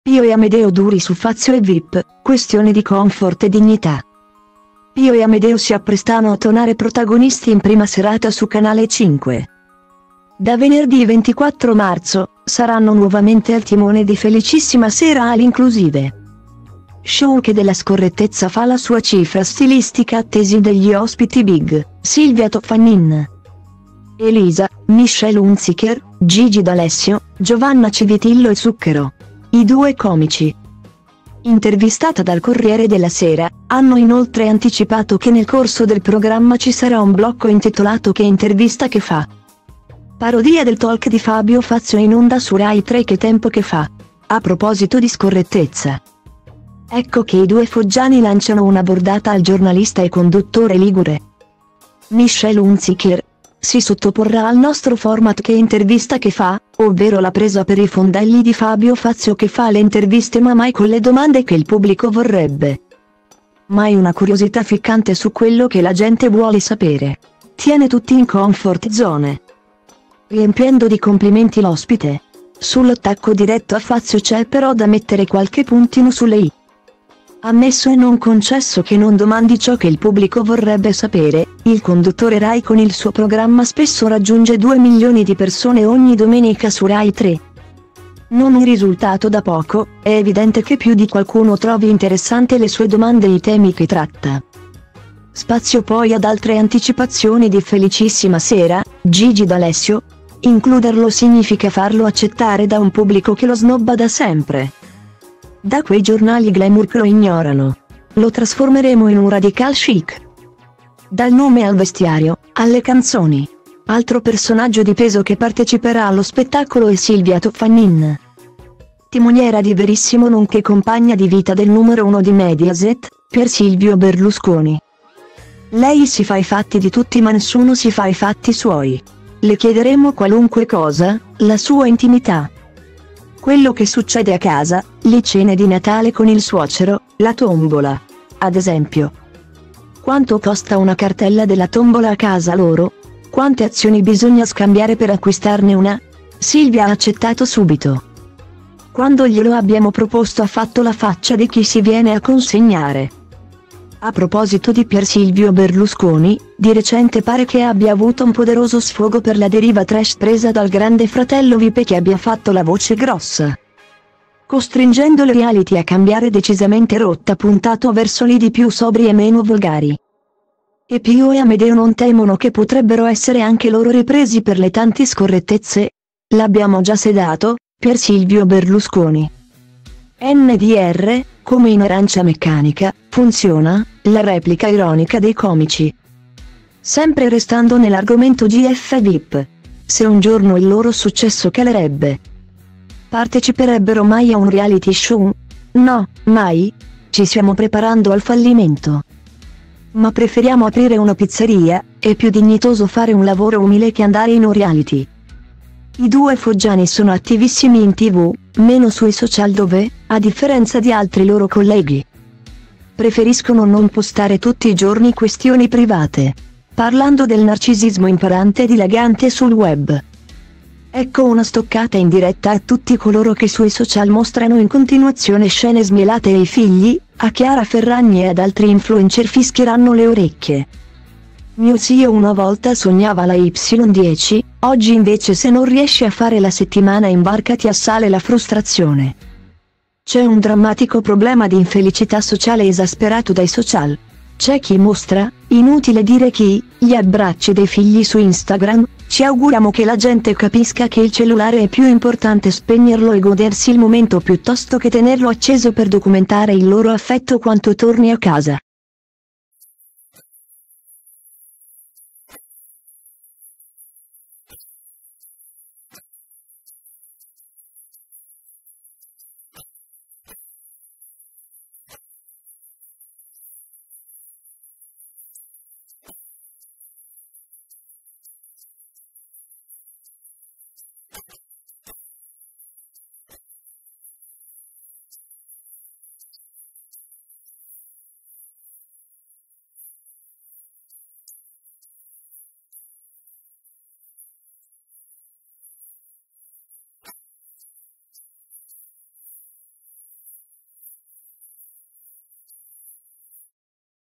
Italian4
Pio e Amedeo duri su Fazio e  Vip: questione di confort e dignità
Pio e Amedeo si apprestano a tonare protagonisti in prima serata su Canale Cinque.Da venerdì 24 marzo, saranno nuovamente al timone di “Felicissima Sera All Inclusive”,show che della “scorrettezza” fa la sua cifra stilistica.   Attesi degli ospiti big: Silvia Toffanin,